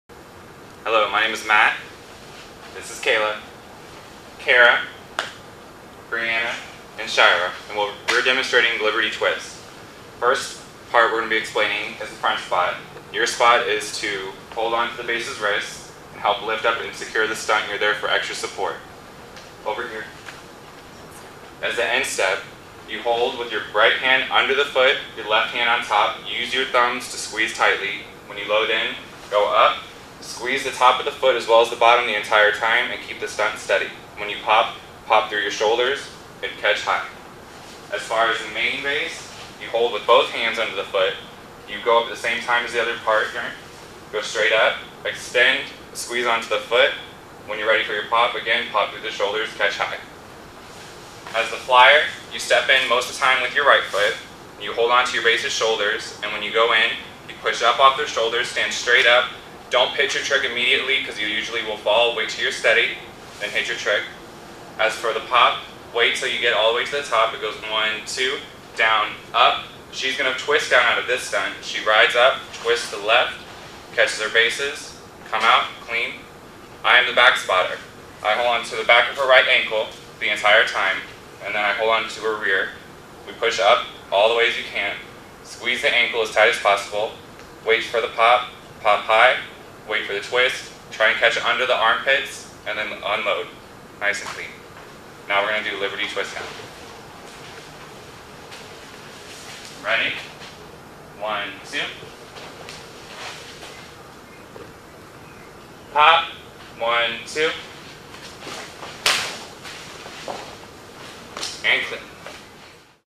How to Do a Liberty Twist
How to Do a Liberty Twist. Part of the series: How to do Advanced Cheer Leading Moves. Learn how to do the liberty twist in this free video series on advanced cheerleading moves.